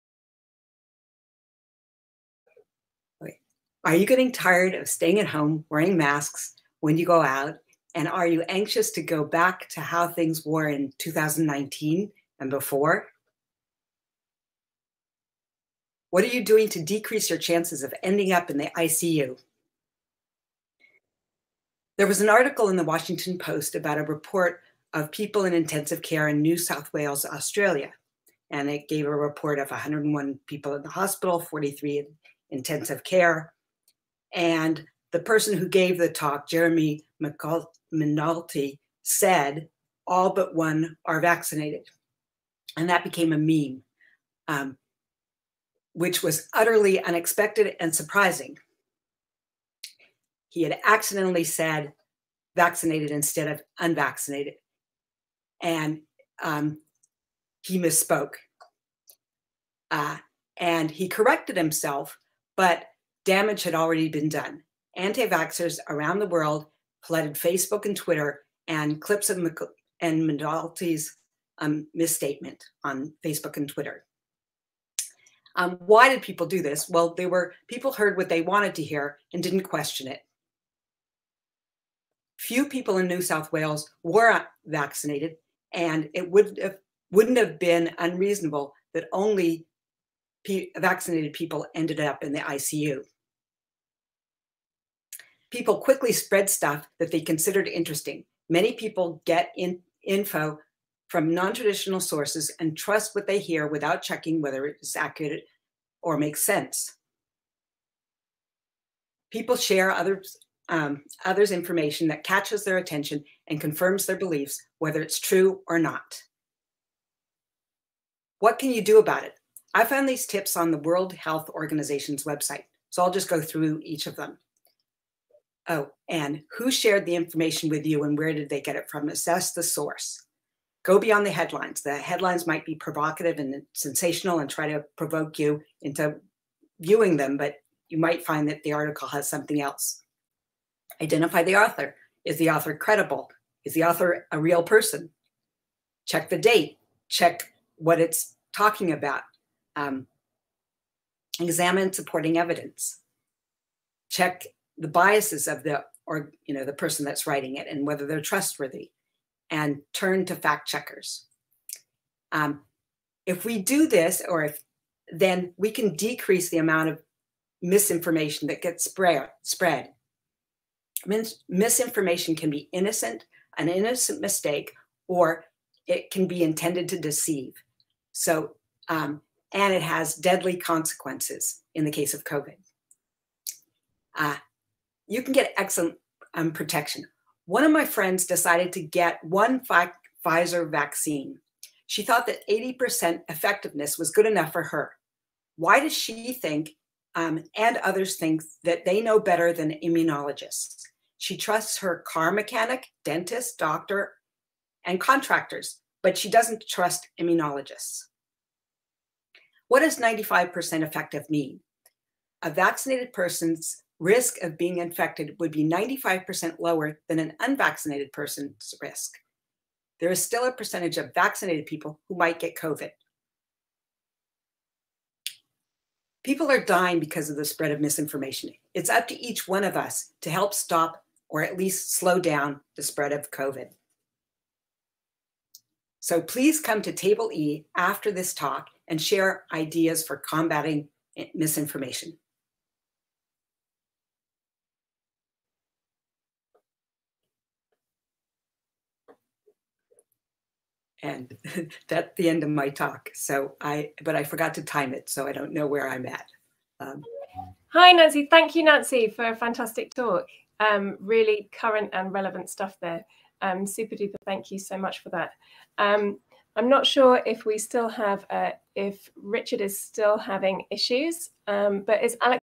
COVID Misinformation Spreads Because Many Don’t Understand Math(s)
Until recently, the general public could get by without understanding probability and statistics. But that changed with COVID-19. Now hundreds of thousands of people haven’t gotten vaccinated and are dying because they don’t understand what they hear or read and social media spreads misinformation rapidly.

Presented at MathsJam November 20, 2021